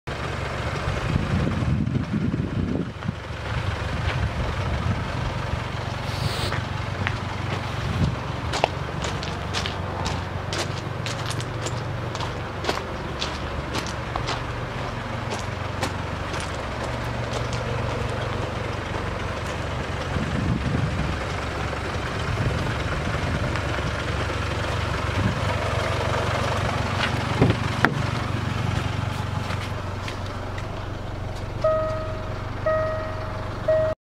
2015(65) Land Rover Range Rover Evoque ED4 SE TECH - Vic Young
Registration: WP65NSE
Mileage: 51,000 miles
Previous Owners: 1
Exterior Colour: WHITE
Engine Size: 2 L
Fuel Type: Diesel
Transmission: Manual
Bodystyle: Estate